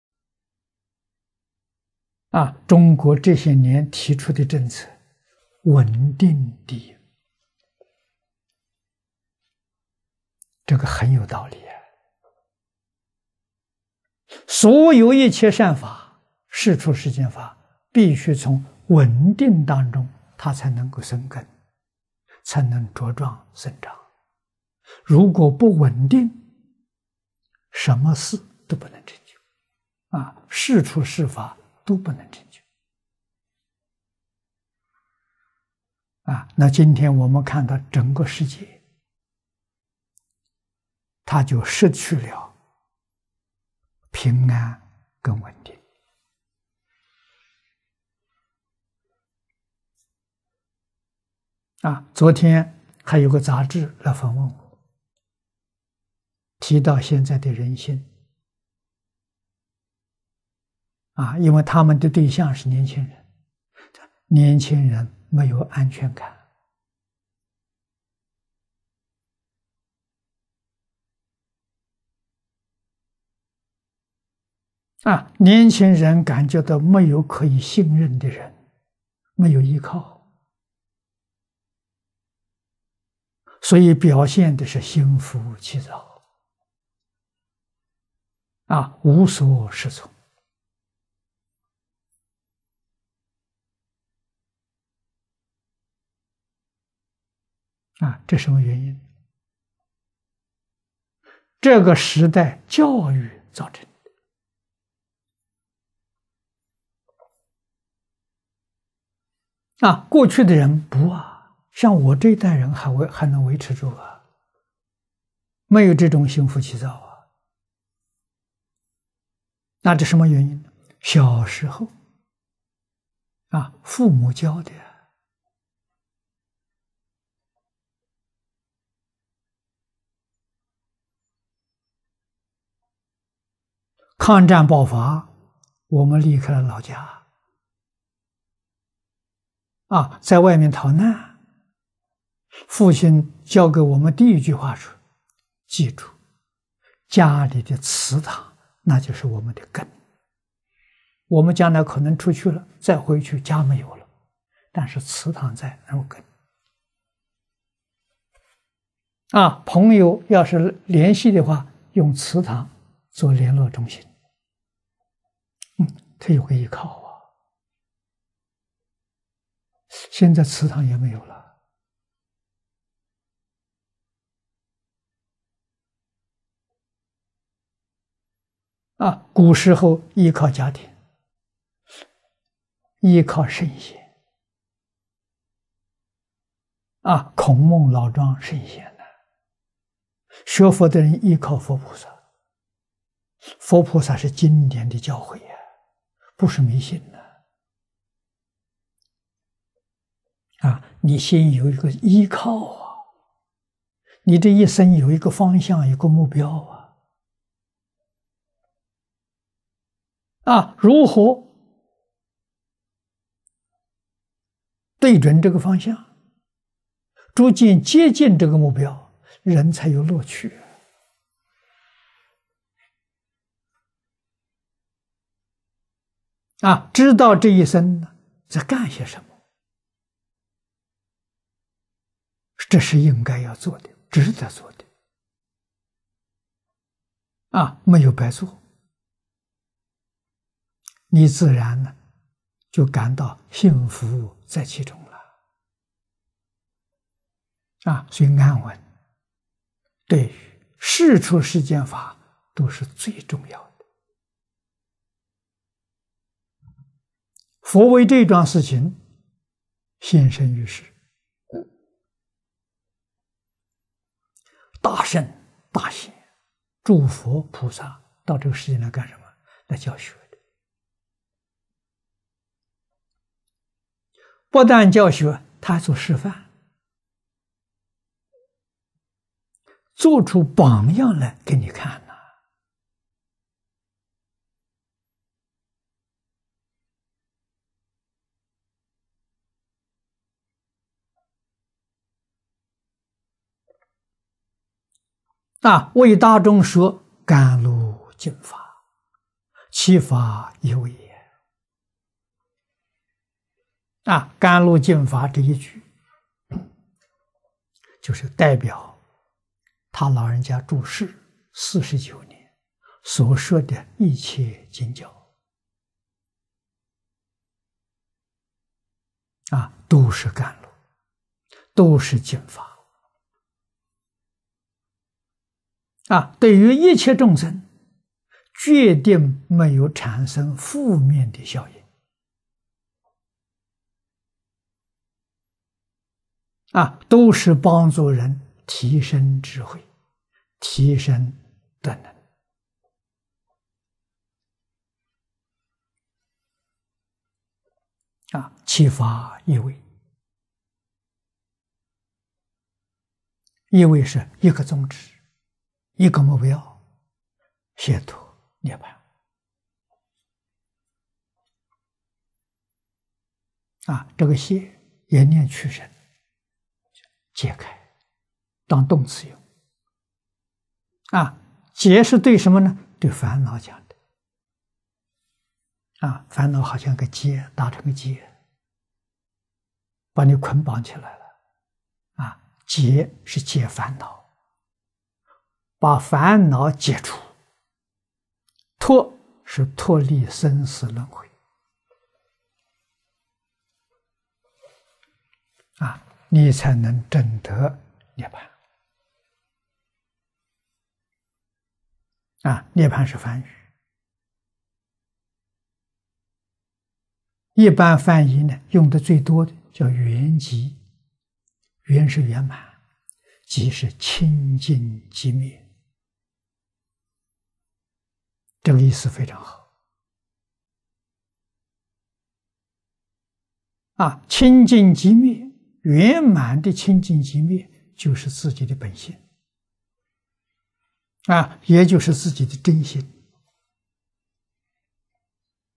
現代社會缺乏穩定
中國這些年提出的政策，穩定第一，這很有道理。所有一切善法，世出世間法，必須從穩定當中它才能夠生根，才能茁壯、生長。如果不穩定，什麼事都不能成就，世出世法都不能成就。今天我們看到整個世界，它就失去了平安跟穩定。昨天還有個雜誌來訪問我，提到現在的人心。因為他們的對象是年輕人，他說年輕人沒有安全感，年輕人感覺到沒有可以信任的人，沒有依靠，所以表現的是心浮氣躁，無所適從。這什麼原因？這個時代教育造成的。過去的人不，像我這一代的人還能維持住，沒有這種心浮氣躁。這什麼原因？小時候父母教的。抗戰爆發，我們離開老家，在外面逃難，父親教給我們第一句話說，記住家裡的祠堂，那就是我們的根。我們將來可能出去了，再回去，家沒有了，但是祠堂在，那是我們根。朋友要是聯繫的話，用祠堂做聯絡中心，他有個依靠。現在祠堂也沒有了。古時候依靠家庭、依靠聖賢，孔孟老莊聖賢，學佛的人依靠佛菩薩，佛菩薩是經典的教誨，不是迷信。你先有一個依靠，你這一生有個方向、有個目標。如何對準這個方向，逐漸接近這個目標，人才有樂趣。知道這一生在幹些什麼，這是應該要做的，值得做的，沒有白做，你自然就感到幸福在其中了。
　　所以安穩，對於世出世間法都是最重要的，佛為這樁事情現身於世。大聖大賢、諸佛菩薩到這個世間來幹什麼？來教學的。不但教學，他還做示範，做出榜樣來給你看。「為大眾說，甘露淨法，其法一味」。甘露淨法這一句，就是代表他老人家住世四十九年所說的一切經教，都是甘露、都是淨法。對於一切眾生，決定沒有產生負面的效應，都是幫助人提升智慧、提升德能。其法一味，一味是一個宗旨、一個目標，解脫涅槃。這個解也念去聲，就是解開，當動詞用。解是對什麼？對煩惱講的，煩惱好像個結，打成個結，把你捆綁起來，解是解煩惱，把煩惱解除。脫是脫離生死輪迴，你才能證得涅槃。涅槃是梵語，一般翻譯用得最多的叫圓寂，圓是圓滿，寂是清淨寂滅，這個意思非常好。清淨寂滅，圓滿的清淨寂滅就是自己的本性，也就是自己的真心。

文摘恭錄—淨土大經解演義（第四七五集）2011/7/3　檔名：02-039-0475